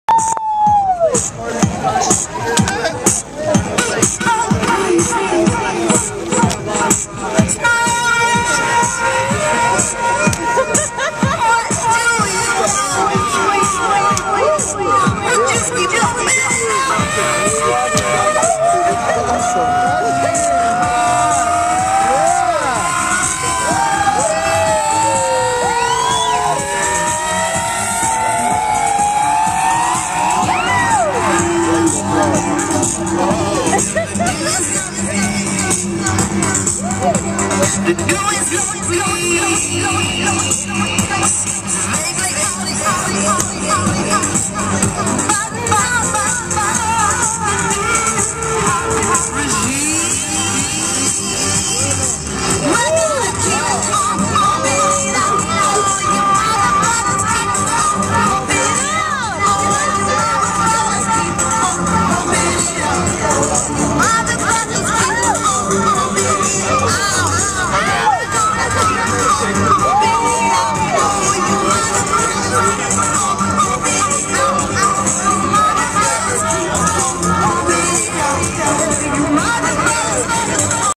Heloise & The Savoir Faire at Phish Superball IX
Late night on Friday (the first night of the festival), walking around the campsites, and this golf cart pulls up out of nowhere and performs what you see on this video.  Had to take a video because I don't think I could have described that happened accurately, and I doubt anyone would have believed me anyway.